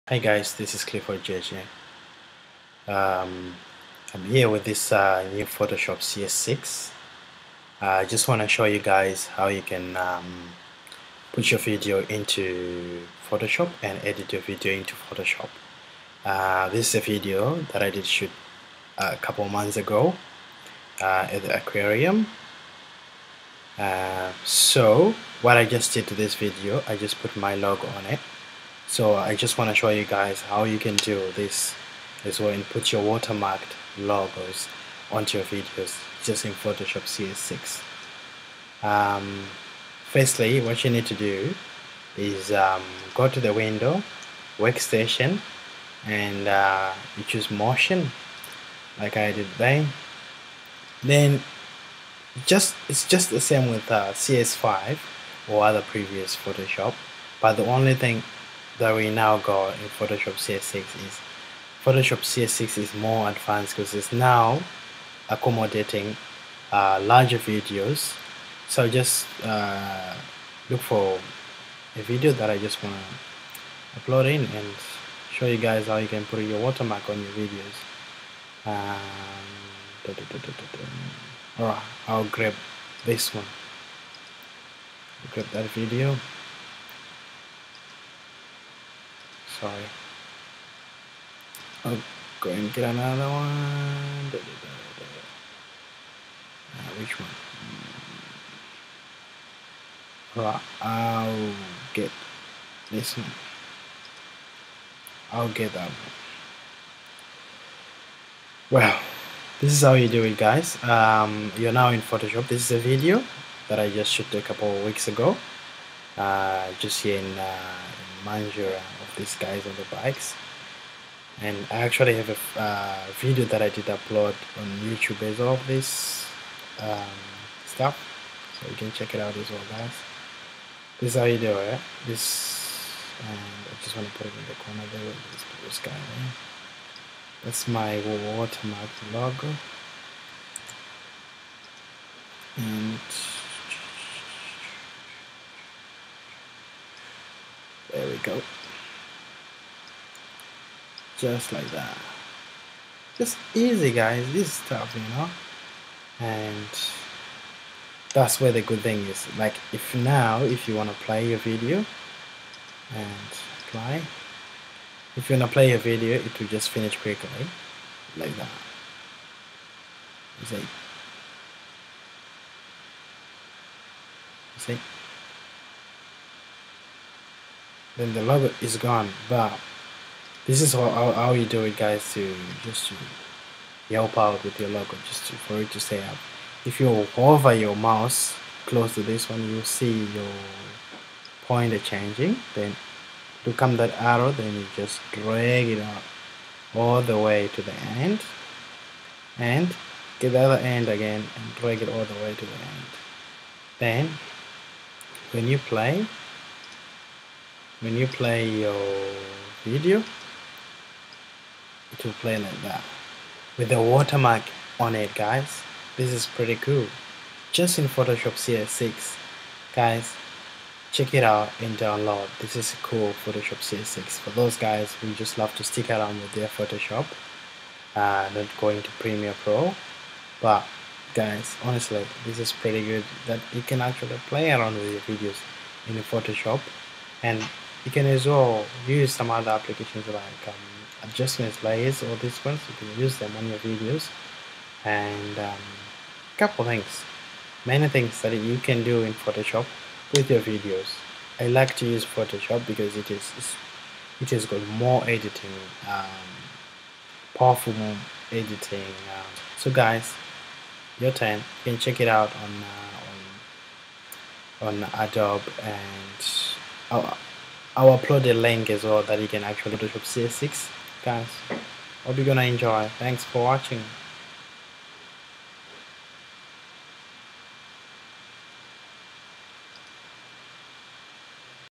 Working with Video in new Photoshop CS6-how to put your watermark on your videos..?
A photoshop tutorial about how to add a watermark into your videos to protect your work.